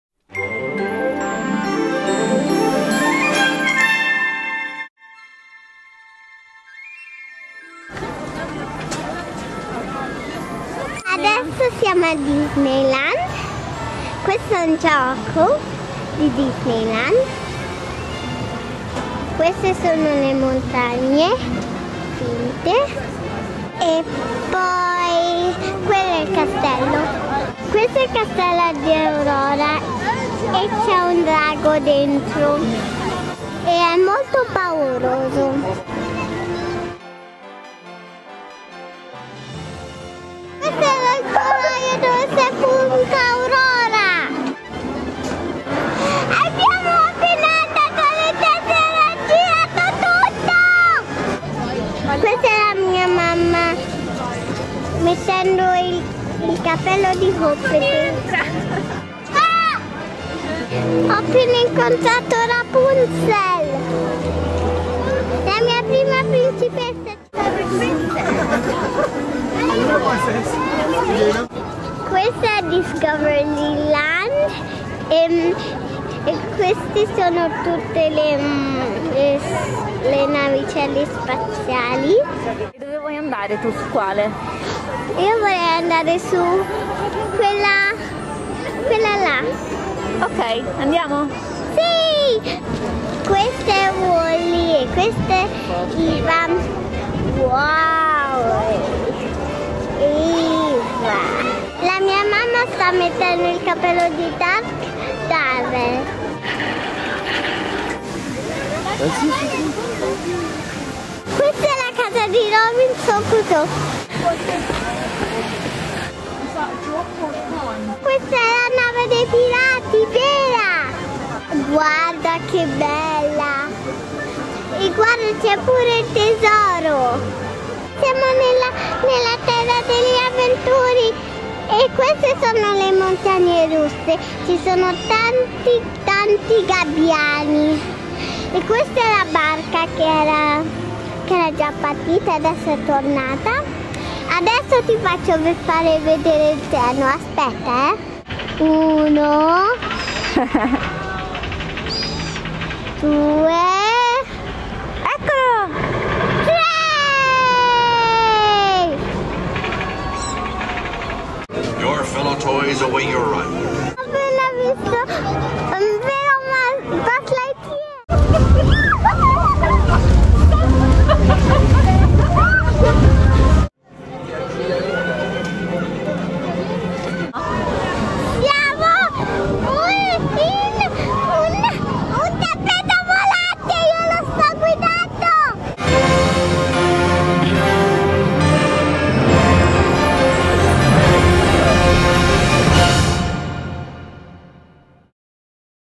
Disneyland Paris raccontata da Viola, 6 anni
Viola, 6 anni, racconta la sua esperienza a Disneyland Paris per disneyxmas, la stagione di Natale 2014.